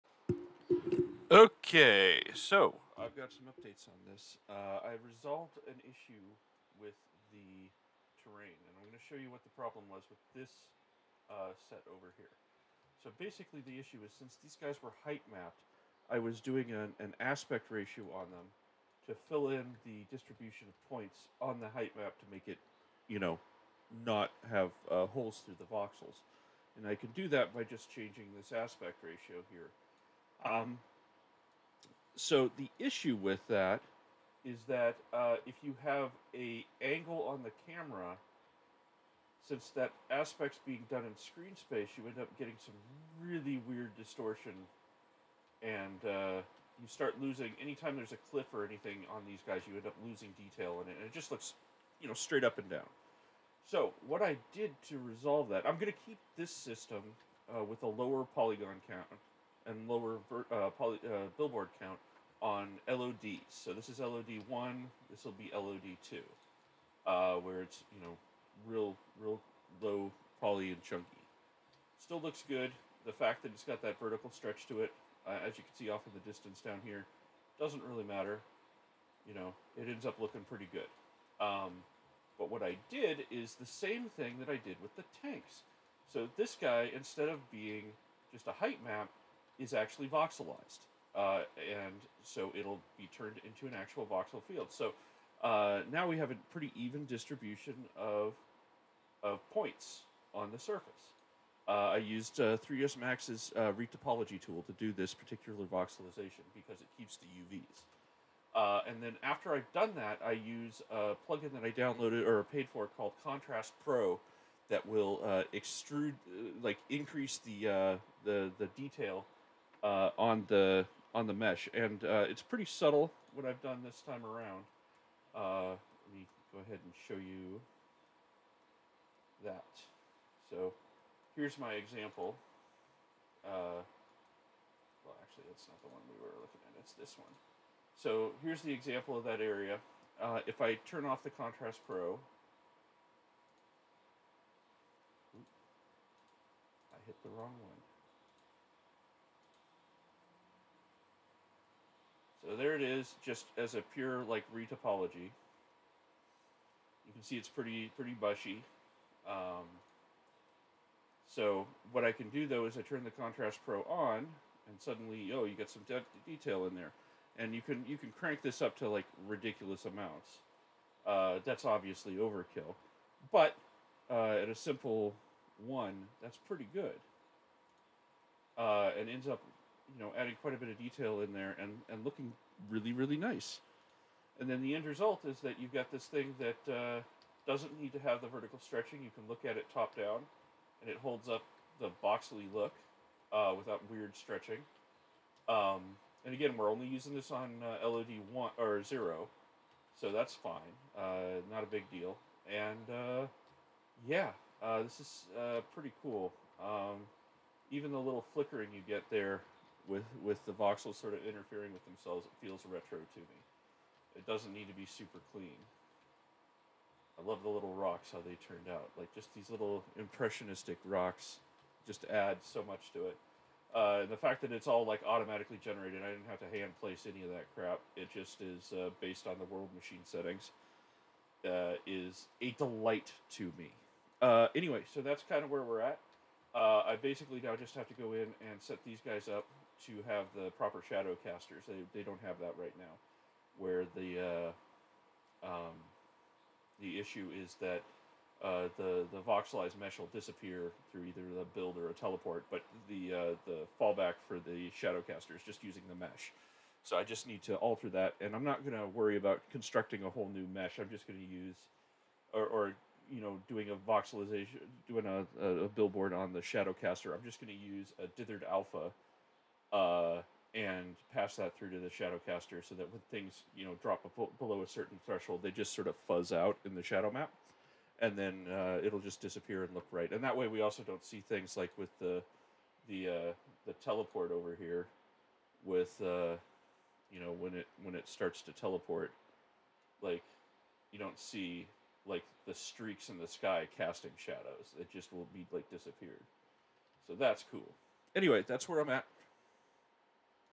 Mars Voxel Tank Terrain shader update!
More voxel side-project progress! Got custom lighting and shadowing working, and I really like how its looking! I've got to get the custom shadow casters for the animated voxel meshes working, and there's some oddness with the camera angle and the terrain voxels...And in this video I show how I solved that problem, for the highest LOD, anyway.
Made in Unity3d, 3dsMax and World Machine!